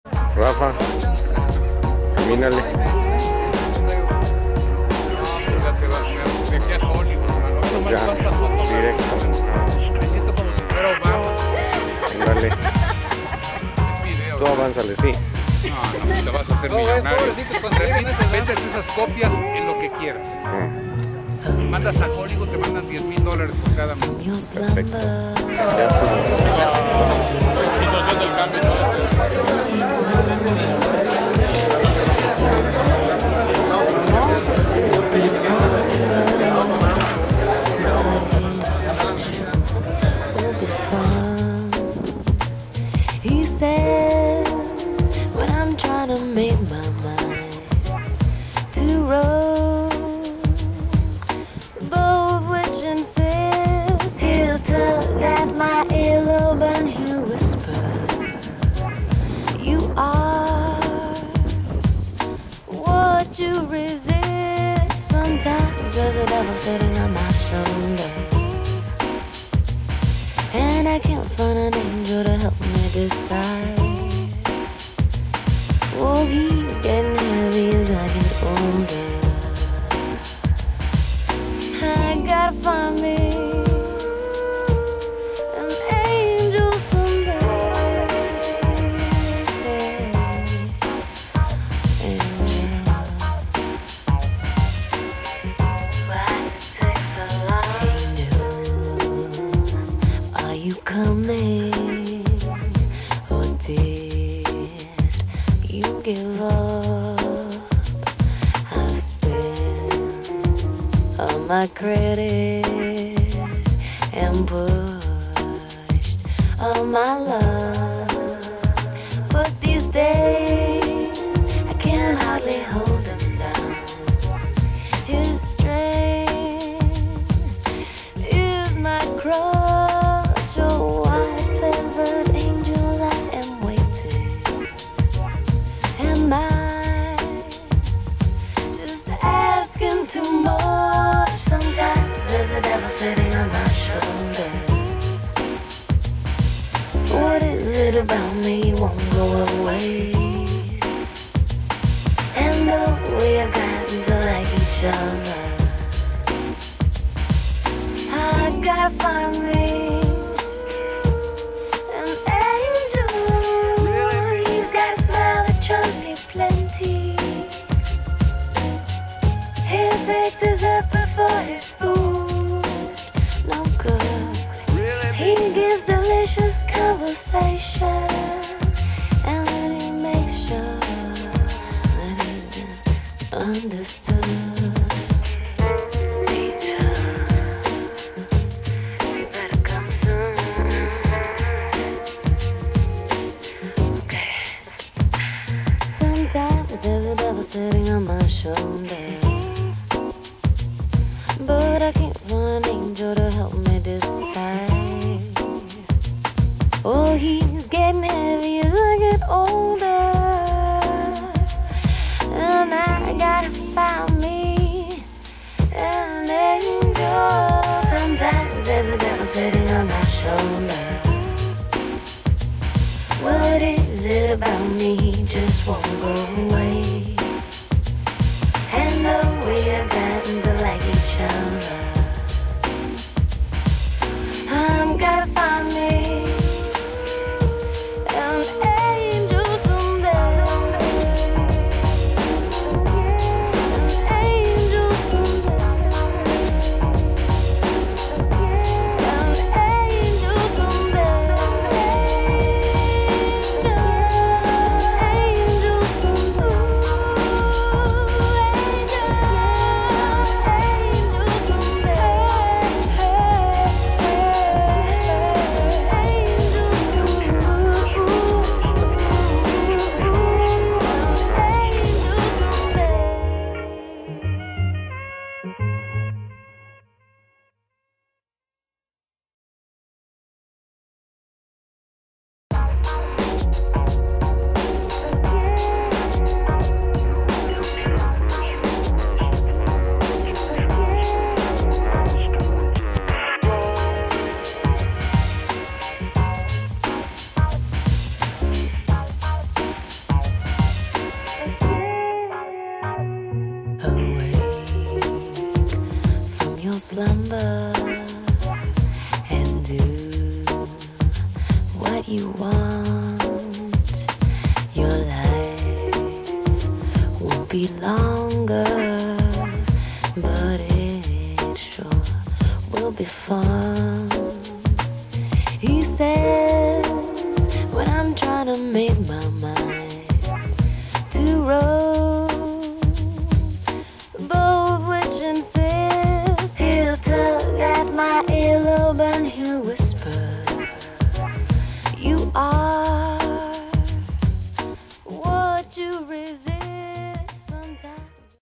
Radioaficionados en Querétaro 2010
Radioaficionados en la Comida en Queretaro 2010 Fotos de XE1KKK y de XE1XWS